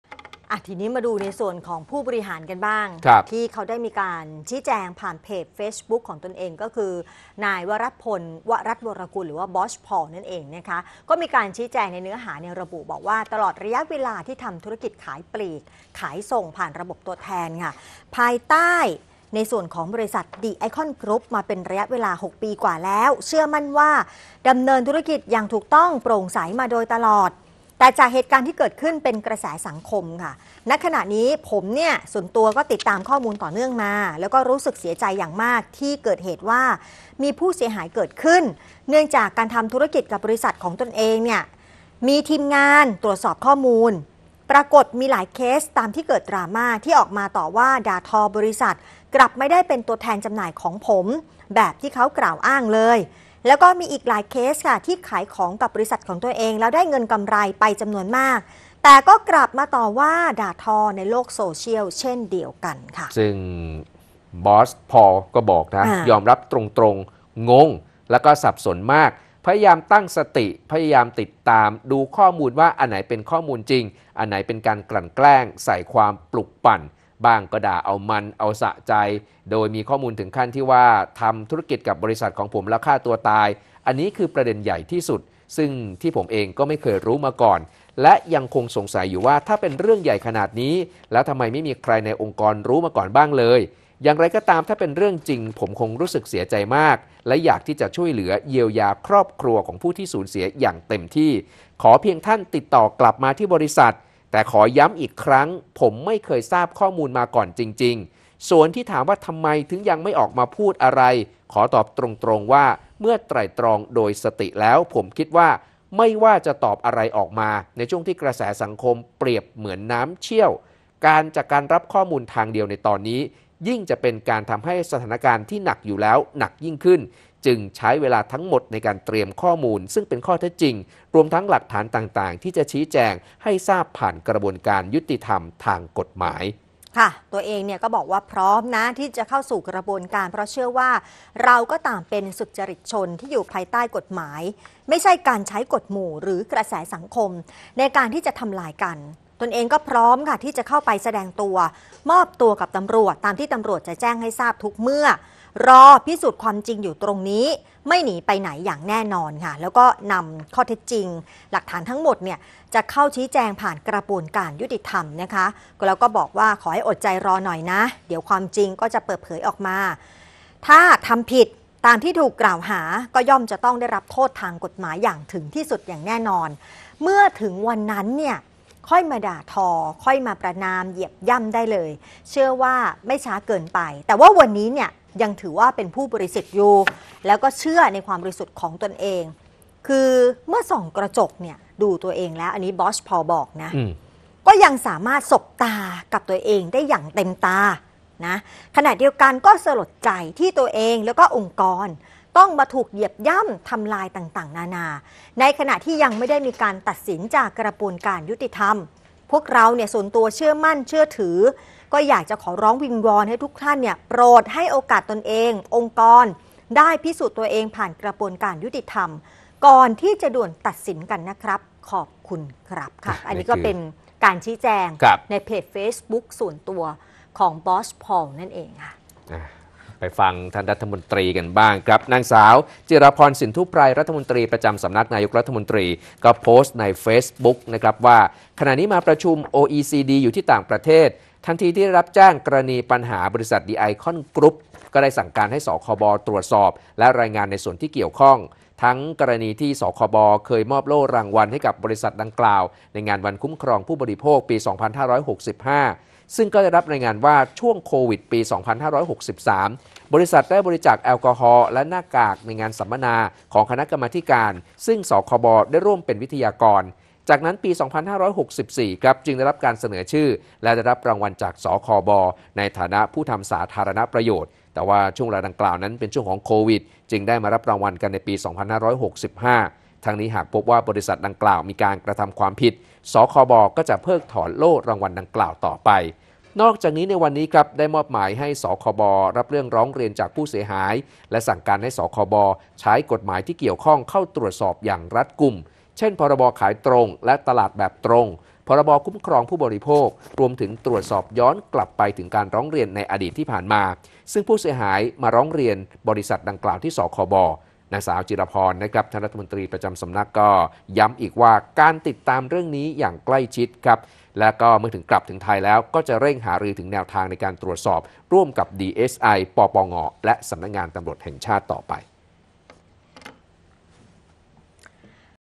"บอสพอล" แจงดราม่า THE iCON / รมต.จิราพร สั่ง สคบ. ตรวจสอบ ข่าวเที่ยง วันที่ 10 ตุลาคม 2567
เฟซบุ้คเพจของ วรัตน์พล วรัทย์วรกุล บอส พอล  โพสต์ข้อความชี้แจง เนื้อหาระบุว่า  ตลอดระยะเวลาที่ทำธุรกิจขายปลีก-ขายส่ง ผ่านระบบตัวแทน ภายใต้ บริษัทดิไอคอนกรุ๊ป มาเป็นระยะเวลา 6 ปีกว่าแล้ว / เชื่อมั่นว่า…ดำเนินธุรกิจอย่างถูกต้องโปร่งใสมาโดยตลอด แต่จากเหตุการณ์ที่เกิดเป็นกระแสสังคมขึ้น ณ ขณะนี้ ผมติดตามข้อมูลต่อเนื่องมา
และรู้สึกเสียใจอย่างมากที่เกิดเหตุว่ามีผู้เสียหายเกิดขึ้น เนื่องจากการทำธุรกิจกับบริษัทของผม / ผมได้ให้ทีมงานตรวจสอบข้อมูล / ปรากฏมีหลายเคสตามที่เกิดดราม่า / ที่ออกมา ต่อว่า ด่าทอบริษัท/ กลับไม่ได้เป็นตัวแทนจำหน่ายของผมแบบที่เค้ากล่าวอ้างเลย /และมีอีกหลายเคสที่ ขายของ กับบริษัทผม แล้วได้เงินกำไรไปจำนวนมาก แต่ก็กลับมาต่อว่า ด่าทอ ในโลกโซเชียลเช่นเดียวกัน